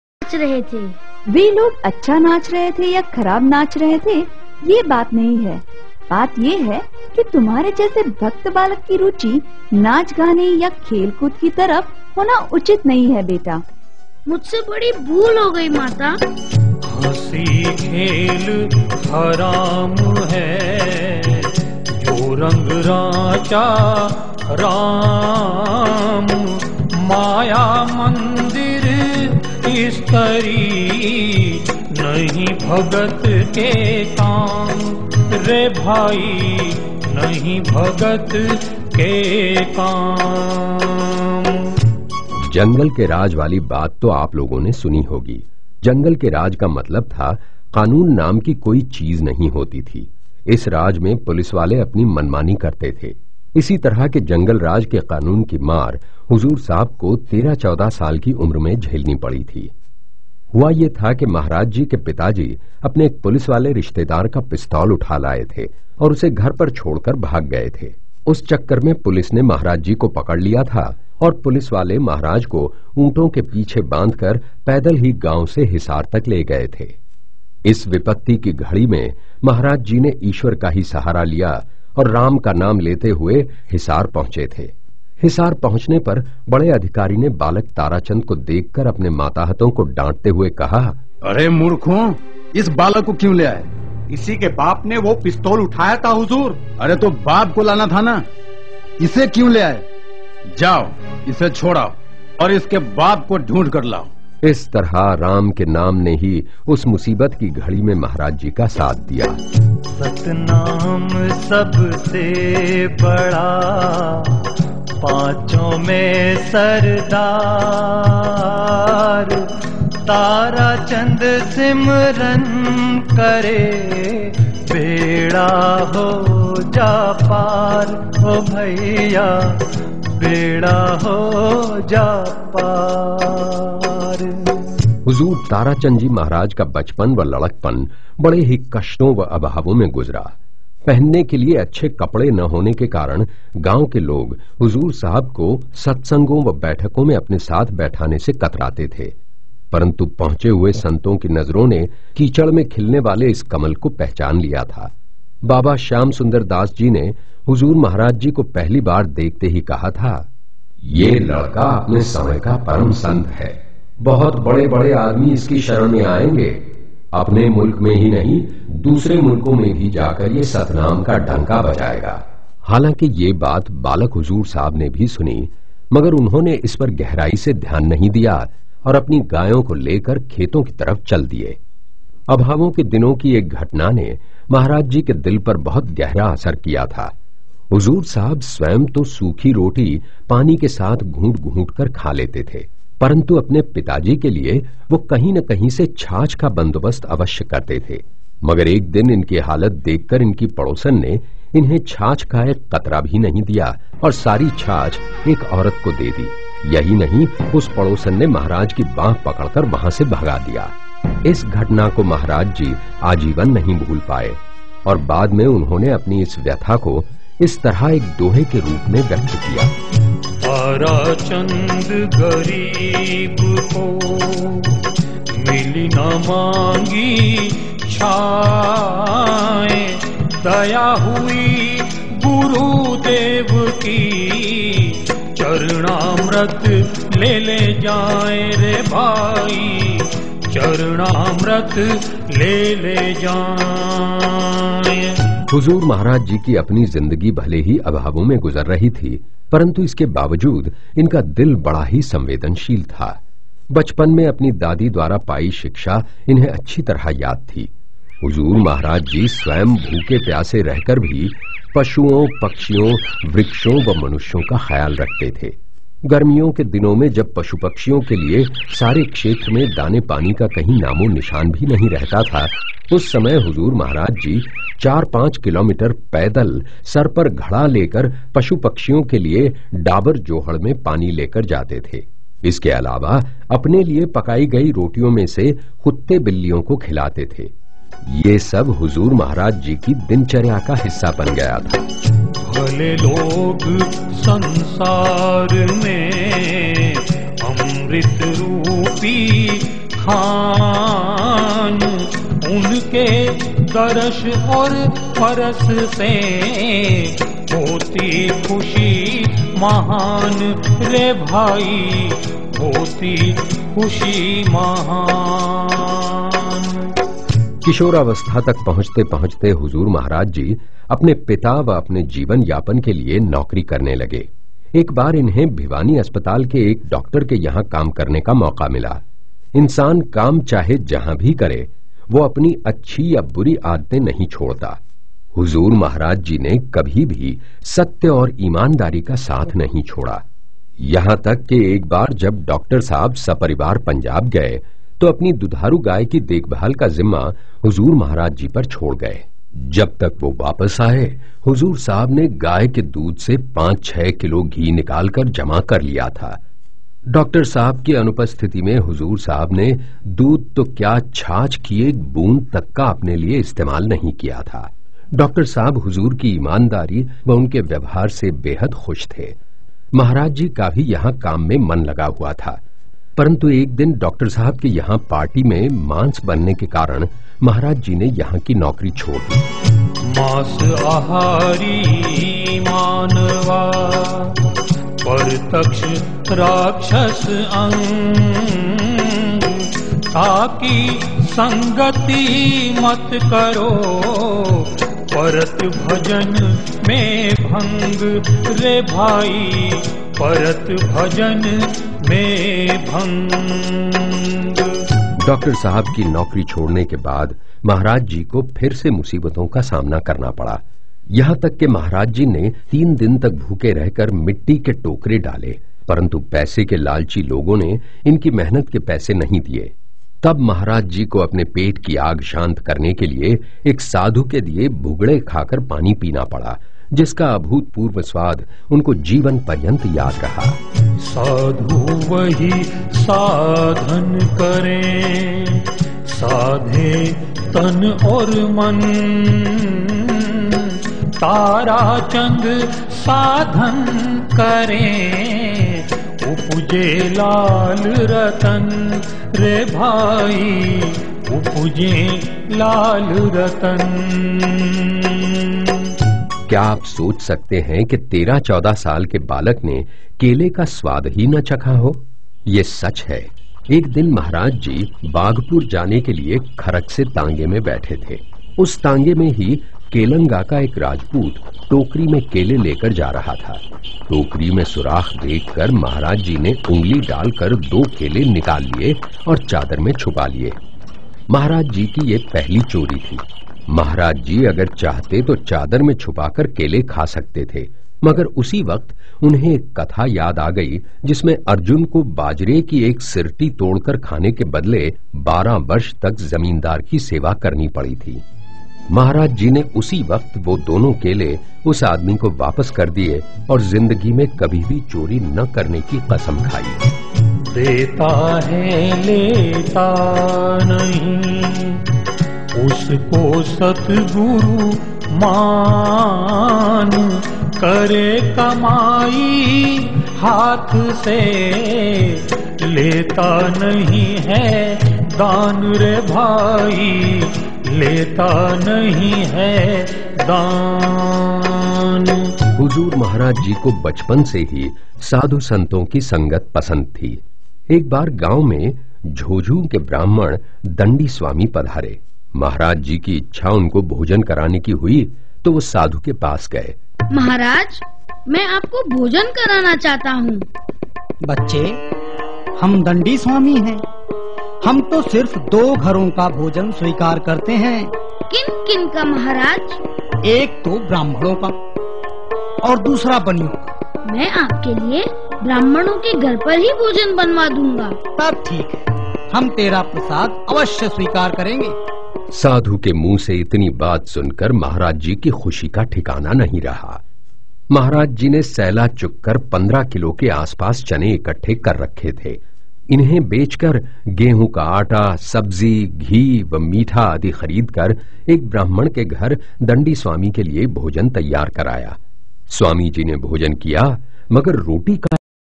RadhaSwami - Life and Teachings of Bade Maharaj Ji. Part 3 of 10.
|| Radha Swami | Radha Swami Dayal Ki Daya, Radha Swami Sahay | Radha Swami ||

Life and Teachings (Jeevni) of Bade Maharaj Ji - Param Sant TaraChand Ji Maharaj.
A Divine presentation by HIS GuruMukh Disciple and (Present Master) Haal Huzur Param Sant Kanwar Saheb Ji Maharaj.

सबको राधास्वामी !!